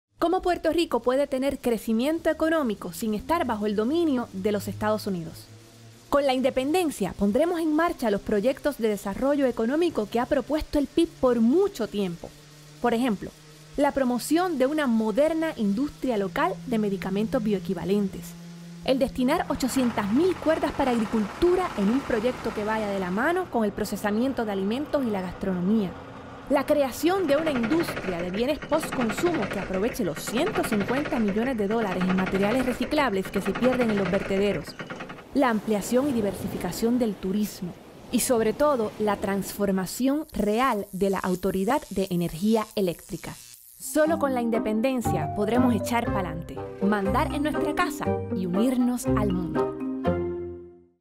¿Cómo Puerto Rico Puede tener Crecimiento Económico sin estar bajo el dominio de USA?
¿Cómo Puerto Rico Puede tener Crecimiento Económico sin estar bajo el dominio de los Estados Unidos?